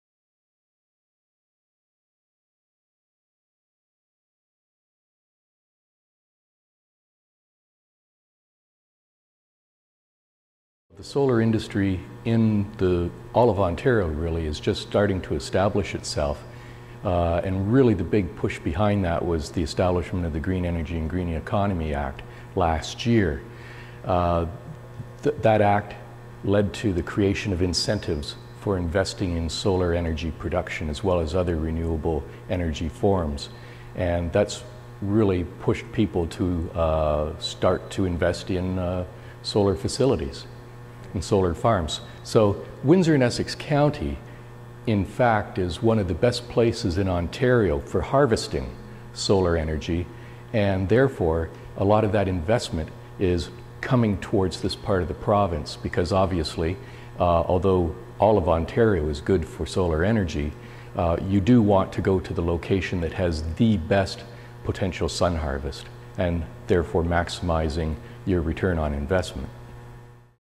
The Potential of Solar Energy in Windsor-Essex
Robin Easterbrook, executive director to the Renewable Energy Technology Center talks with Workforce WindsorEssex about the industry of solar system manufacturing.